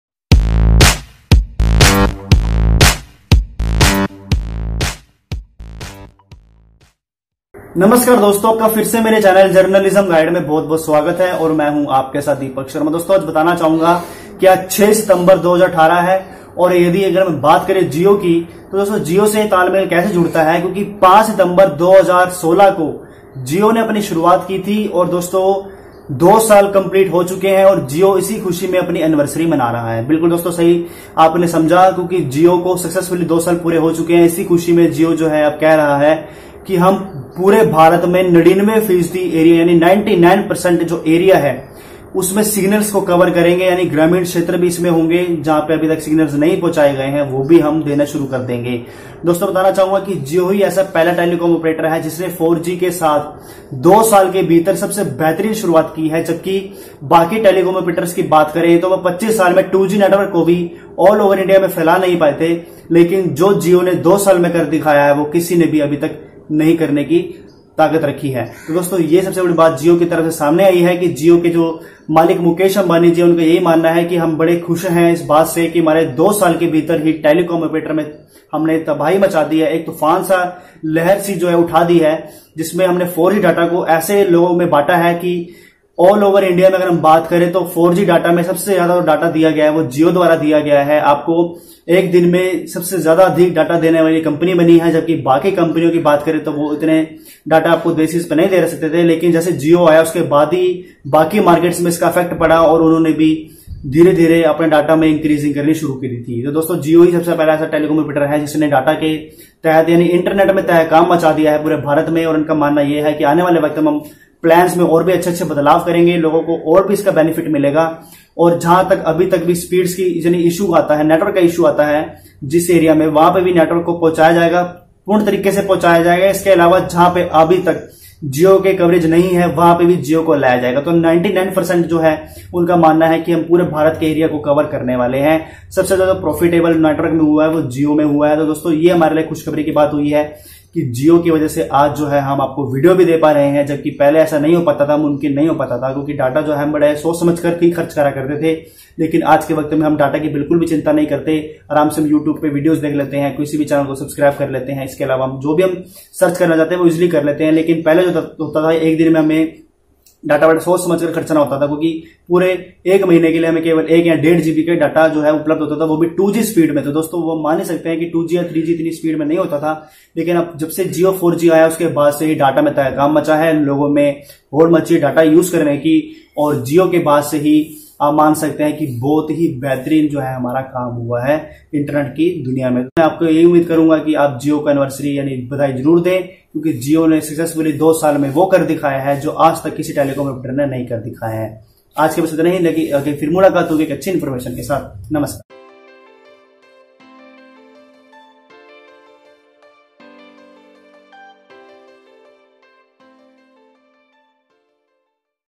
JG Update: Jio Celebrating 2nd Annverisory with its Subscribers, Many New Plans, Offers Coming.(M.W)
Namaskar Dosto,

Jio Ne aaj se 2 saal pure kar liye hai. iski khushi mein jio new plans, offers, more data packs launch karne ja rha hai. iski puri janakri ke liye video ko pura jarur dekhe.

Dhanywad.